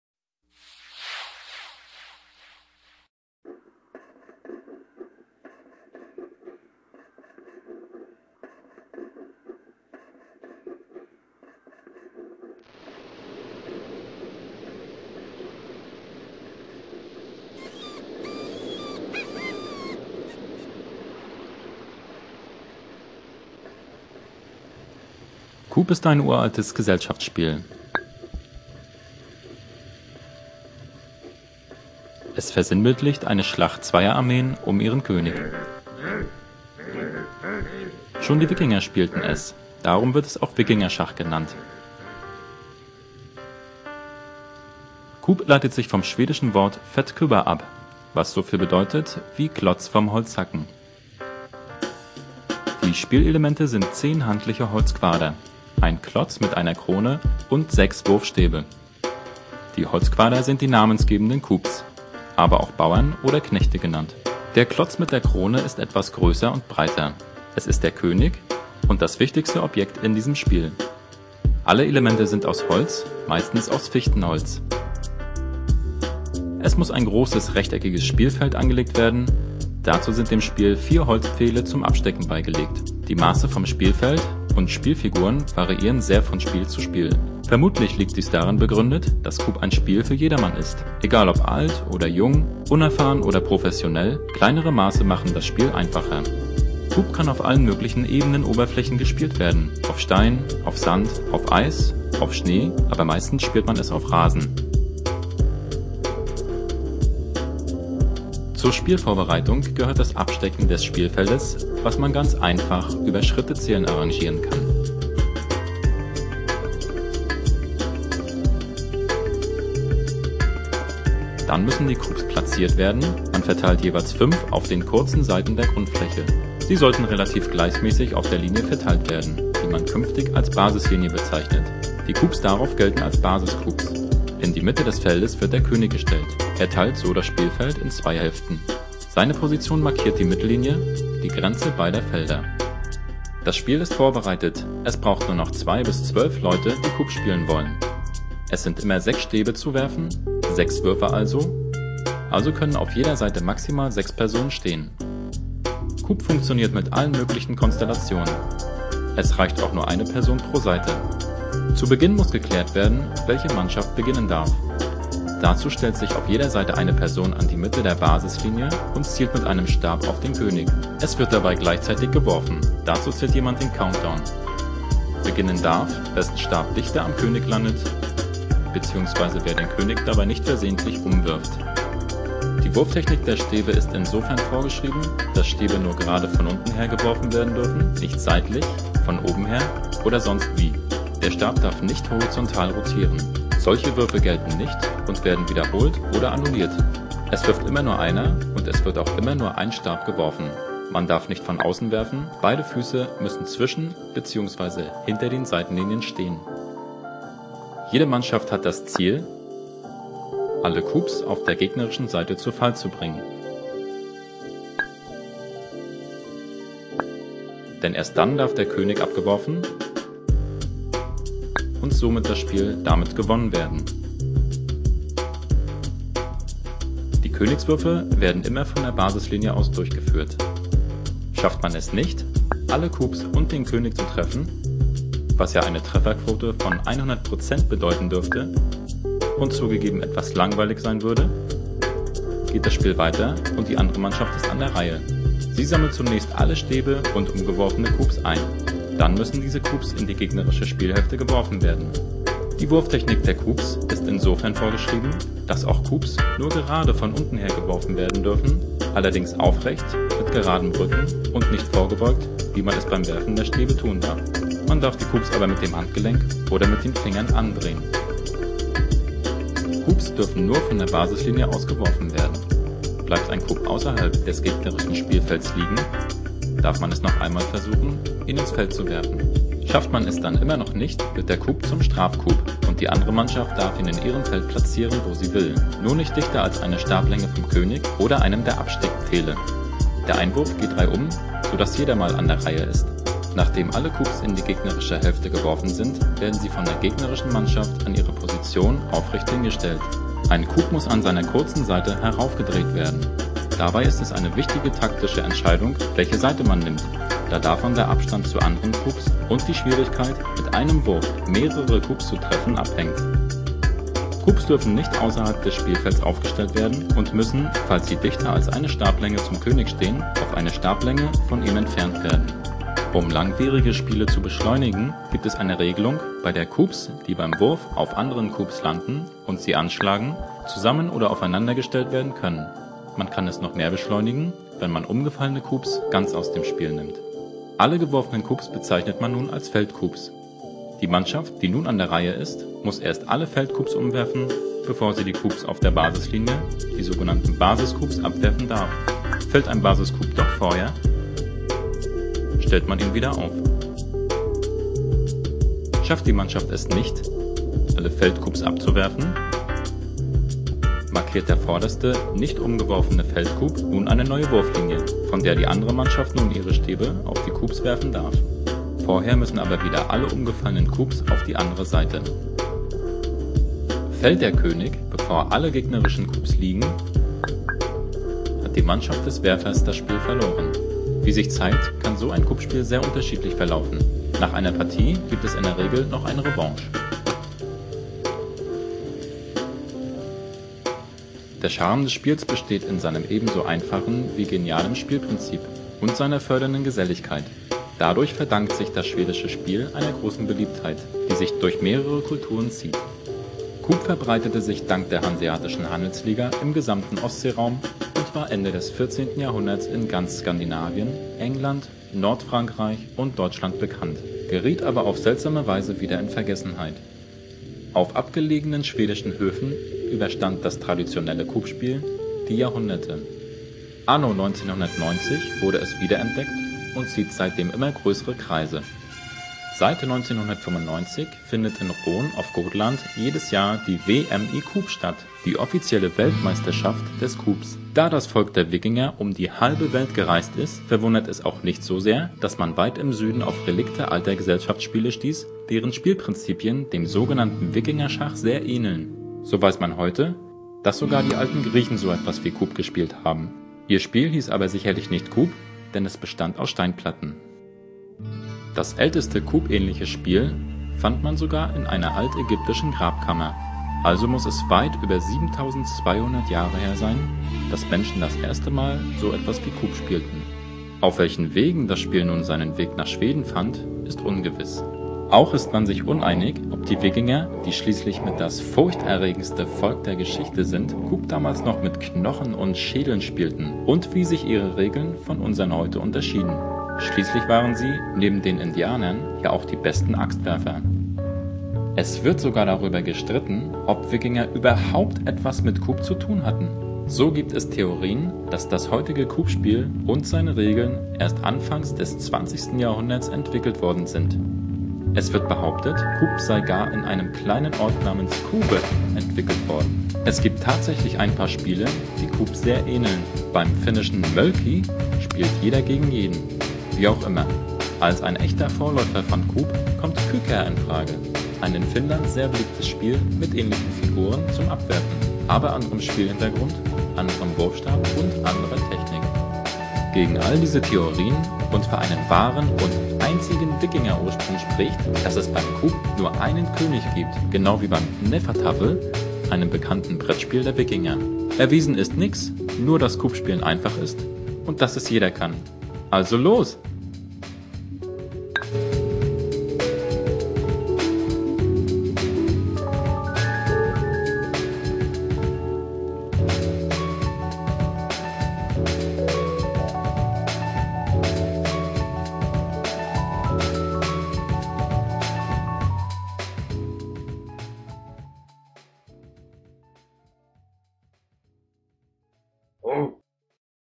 KUBB - Was ist Wikingerschach?
Ein Animations-Film über das beliebte schwedische outdoor-Spiel. Es werden die Regeln erklärt und die Herkunft beleuchtet. Der Film ist ein Studienprojekt für den Kurs CutUp an der Fachhochschule Potsdam.